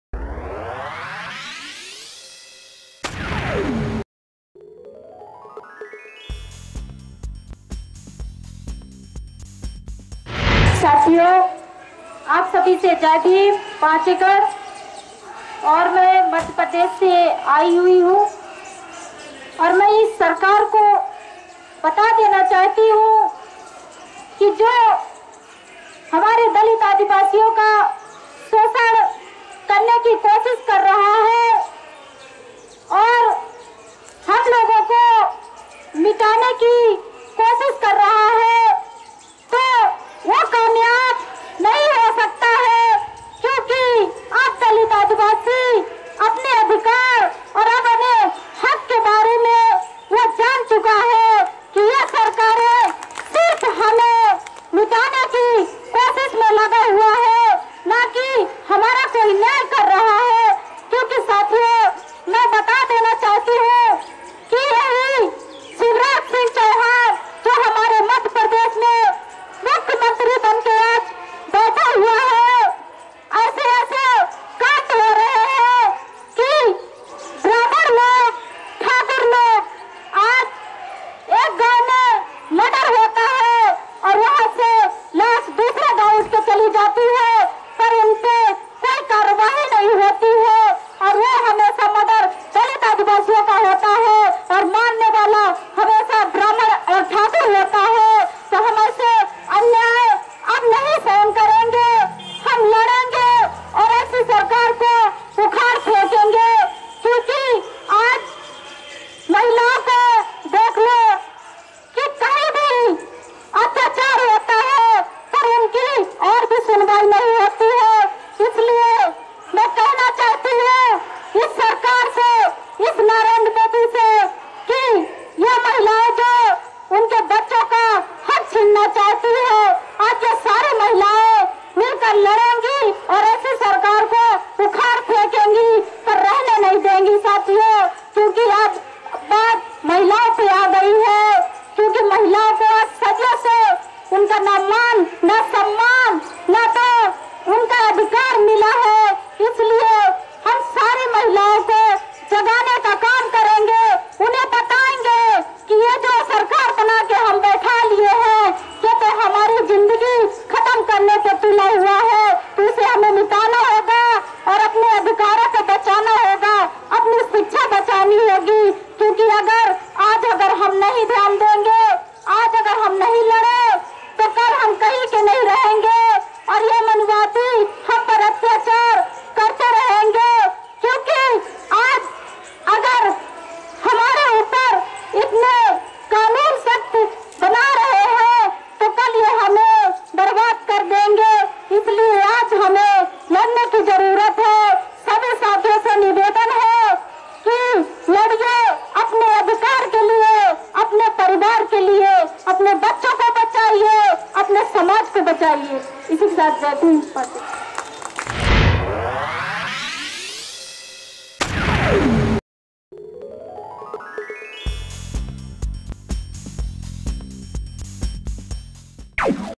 Dalit Adivasiyo को प्रताड़ित करने की सरकार की मंशा हम पूरी नहीं होने देंगे-JAYS Andolan-SM News
Dalit Adivasiyo को प्रताड़ित करने की सरकार की मंशा हम पूरी नहीं होने देंगे